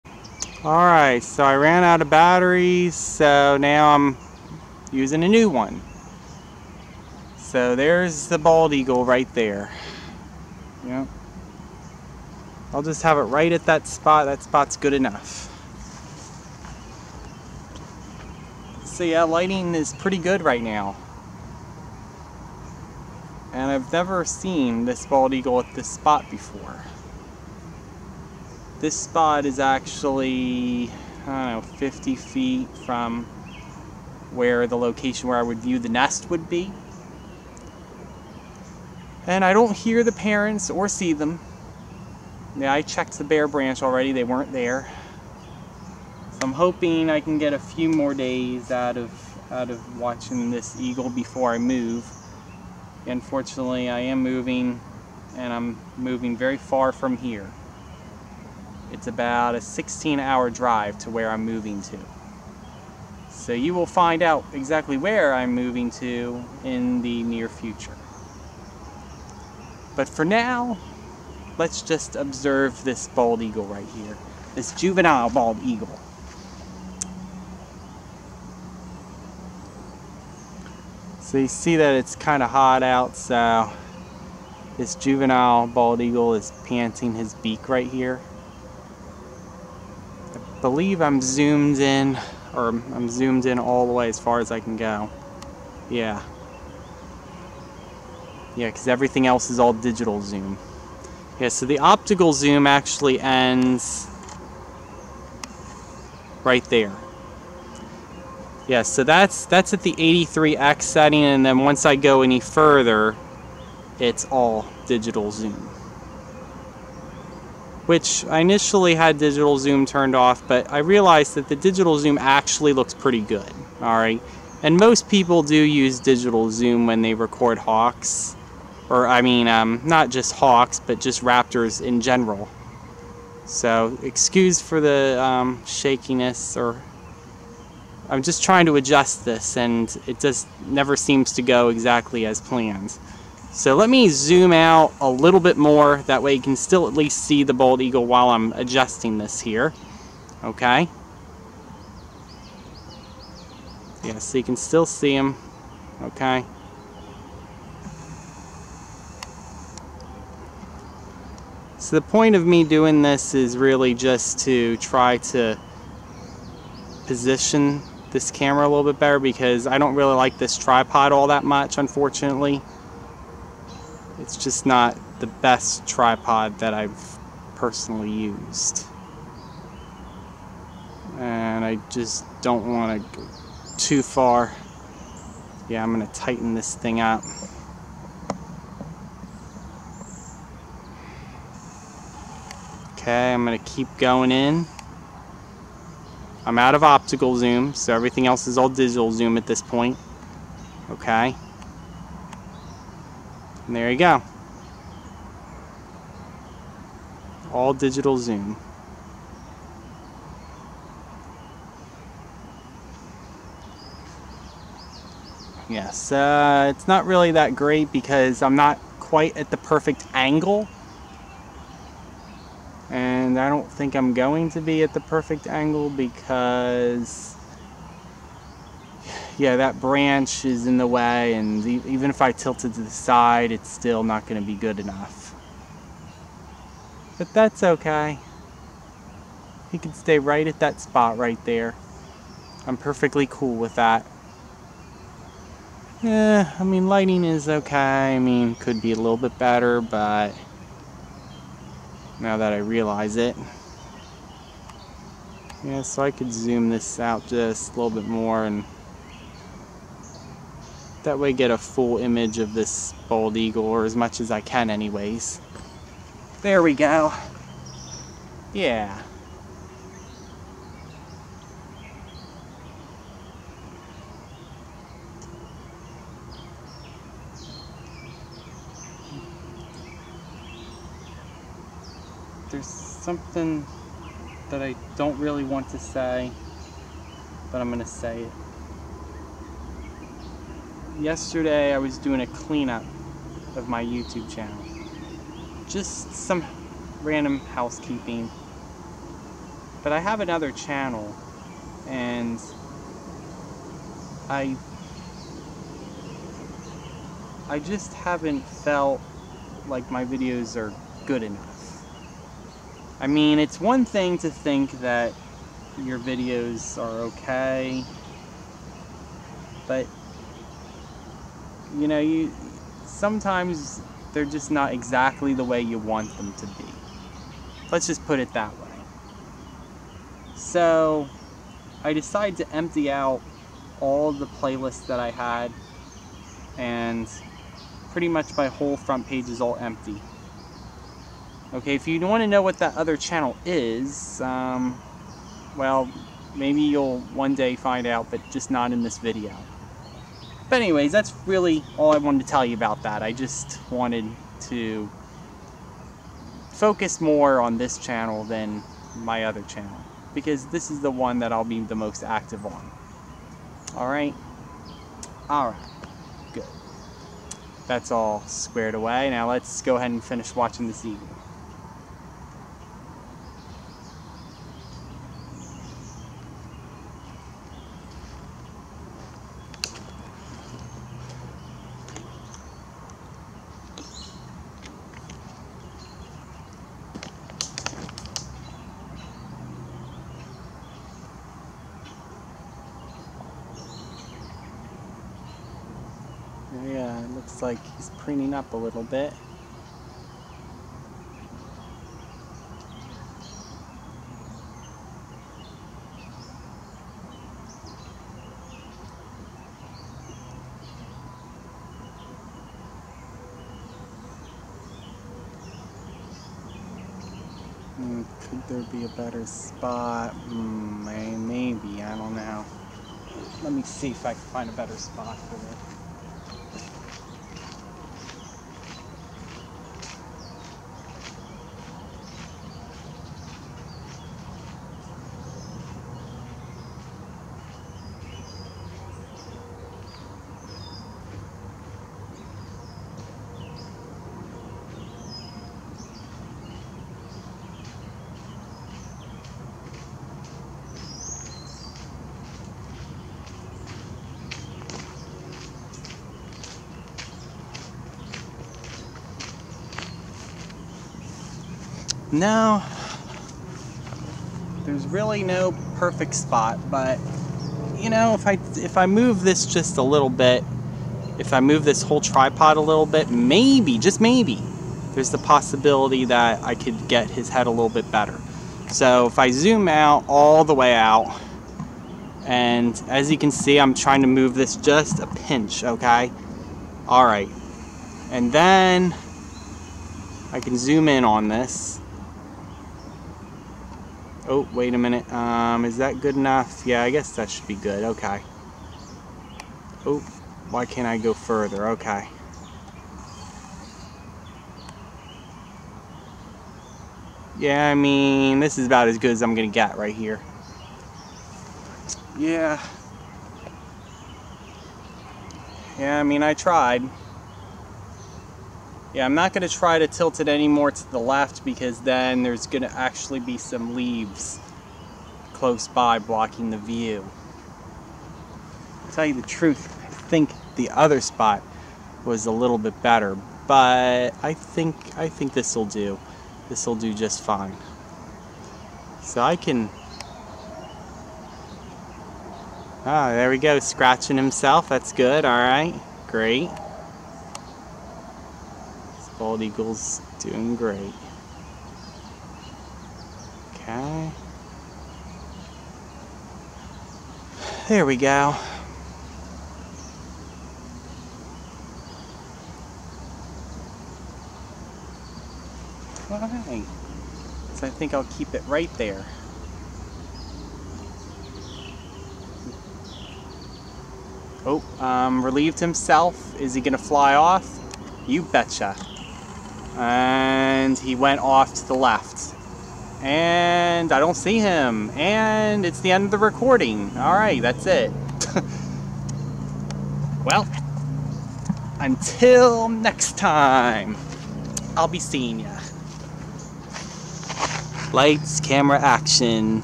Bald Eagle Fledgling on Tree Towpath Trail July 14, 2019
Angoueleven Fleece Blanket Throw Bald Eagle Fledgling Pair Standing ATOP Nest Throw Blanket 90"x70"

As of today, I can now verify that the Bald Eaglet that was in the nest has recently fledged. Here he is seen on a tree next to the nest. That would explain why I haven't seen the family around since July 9th. I know he's doing quite well at hunting prey on his own because he relieved himself right before taking off at the end of the video.